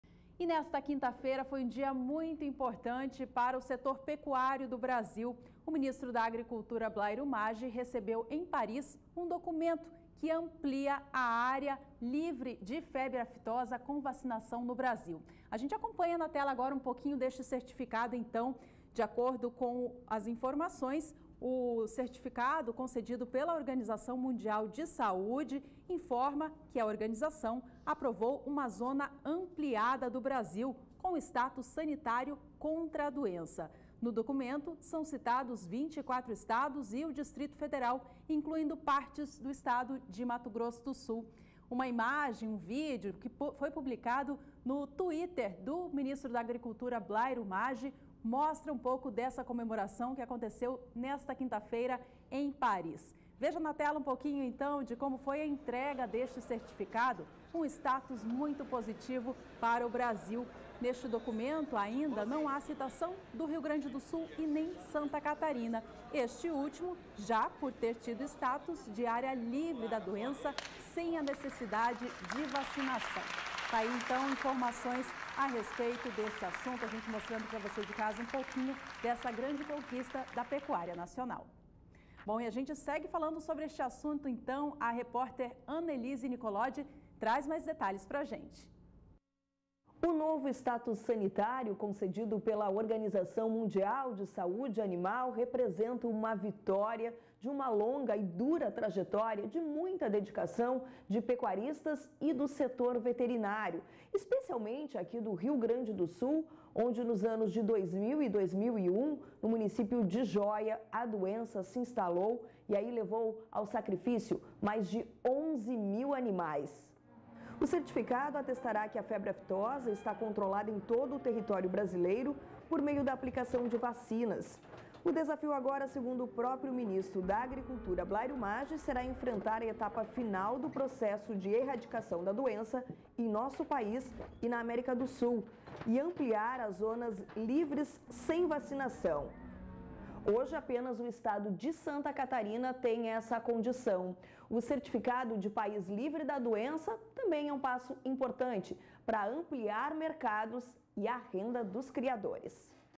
ÁREA LIVRE DE AFTOSA - 25/05/2018
O ministro da Agricultura, Blairo Maggi, recebeu nesta última quinta-feira (24), em Paris, o certificado de ampliação da área livre de febre
aftosa com vacinação no País concedido pela Organização Mundial de Saúde Animal (OIE).

O documento, divulgado pelo próprio ministro, informa que a OIE aprovou "uma zona ampliada do Brasil" com o status sanitário contra a doença. No documento são citados 24 Estados e o Distrito Federal, incluindo "partes de Mato Grosso do Sul".